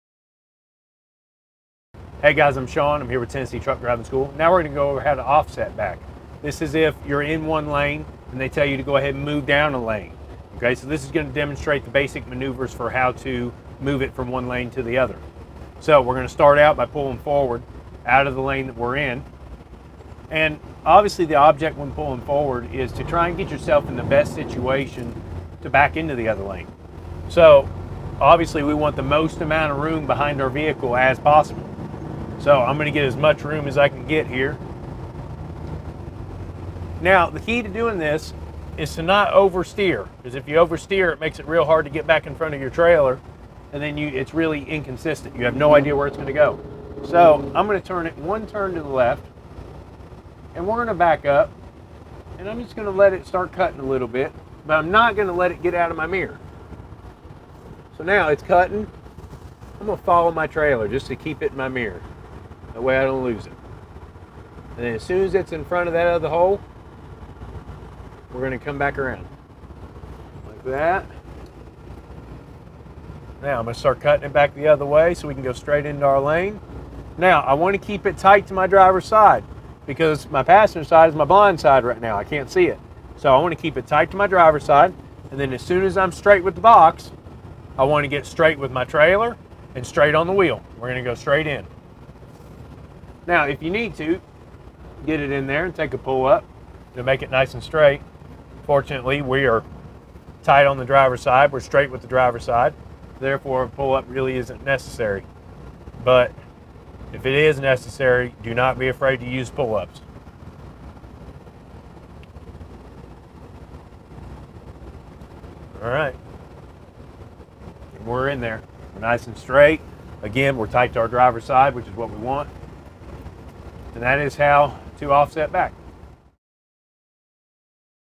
Offset Backing
Sean Henson from Tennessee Truck Driving School shows us how a Offset Backing should be done in order to pass the Tennessee CDL test.